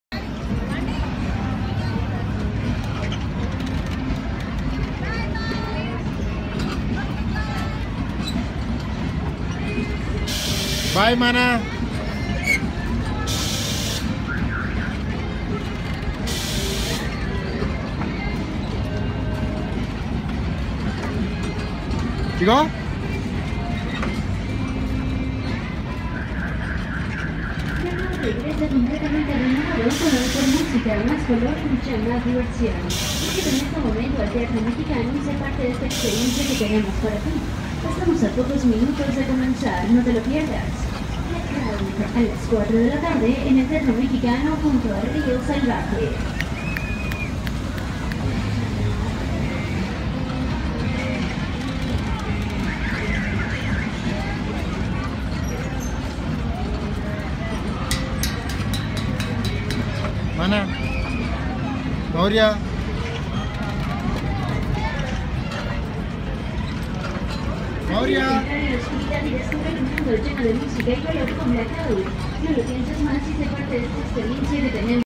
Mourya Teja - Ride 1 @Six Flags, Mexico
Mourya is from Rayaparthy, Warangal, Telangana, India